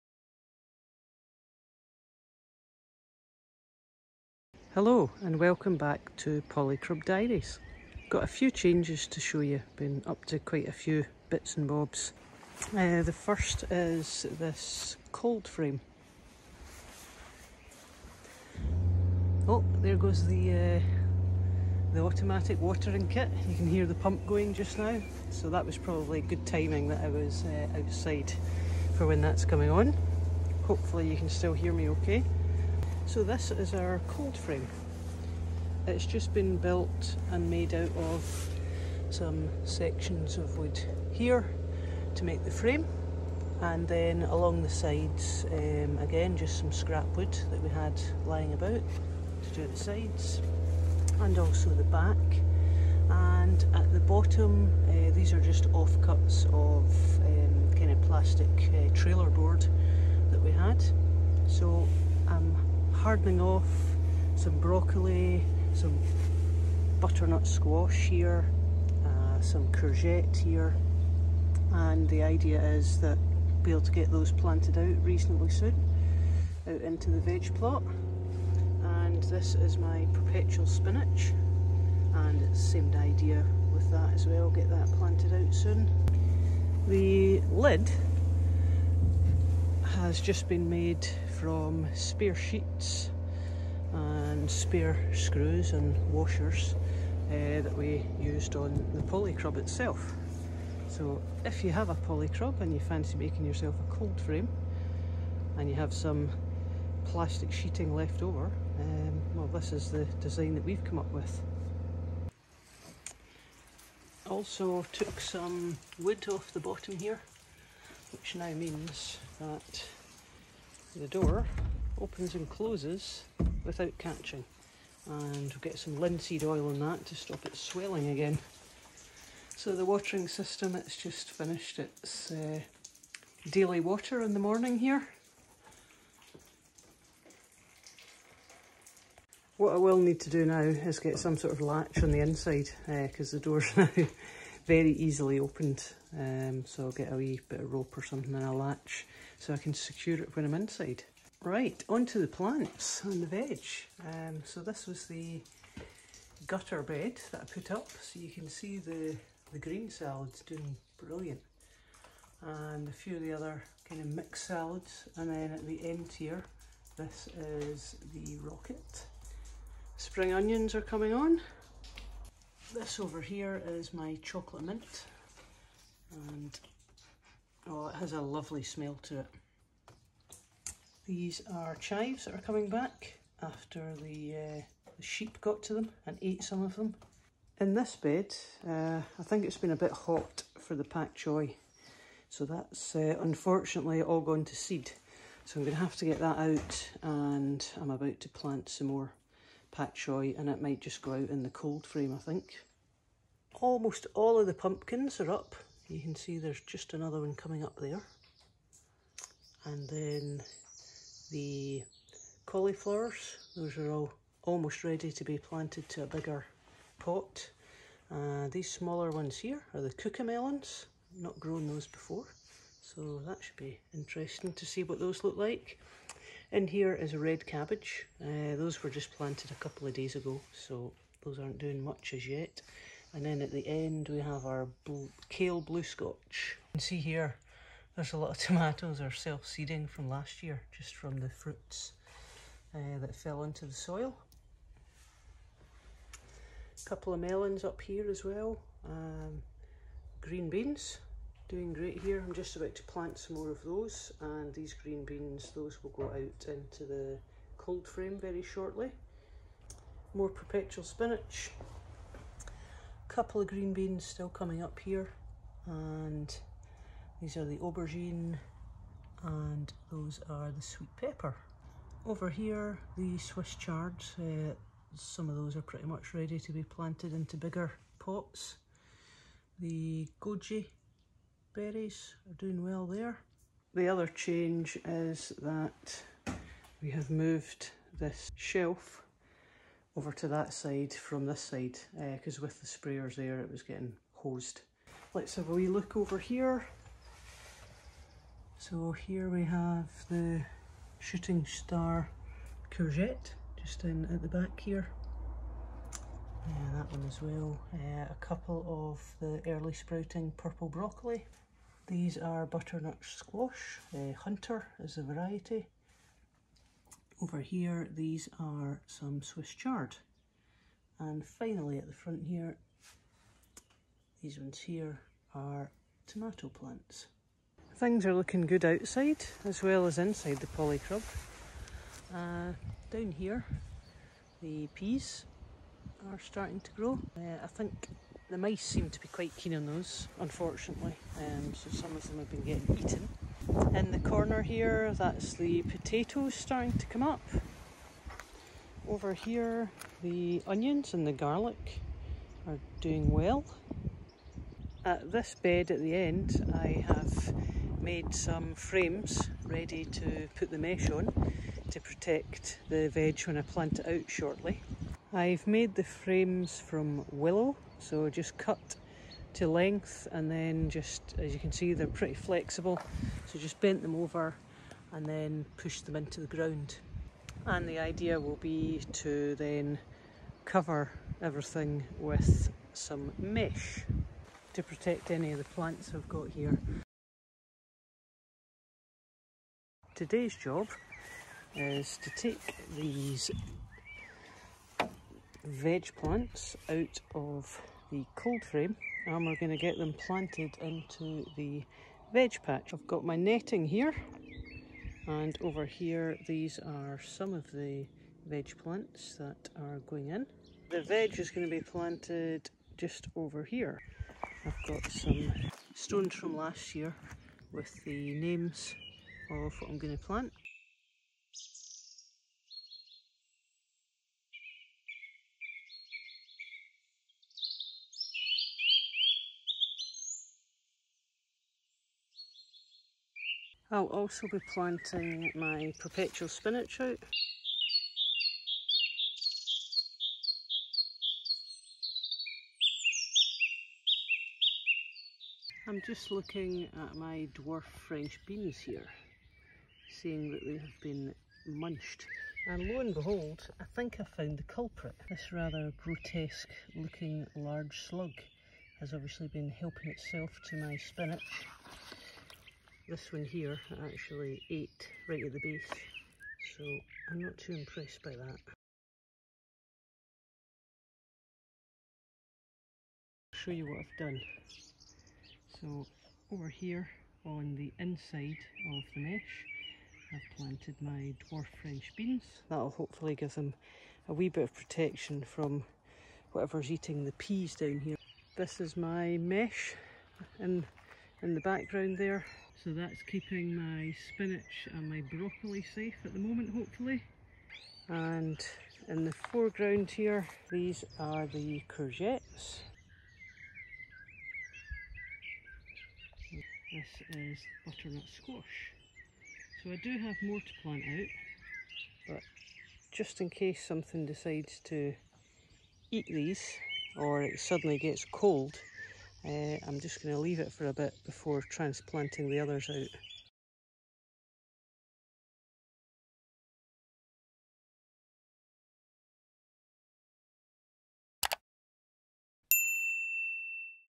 Some changes inside and out - Polycrub and cold frame
I've been making a few changes inside the polycrub, building a cold frame, and planting some seedlings into my veg plot.

Birdsong during the time lapse:

Sound Effect - Nature Sounds
Audio And Video Recorded By Sonny Fascia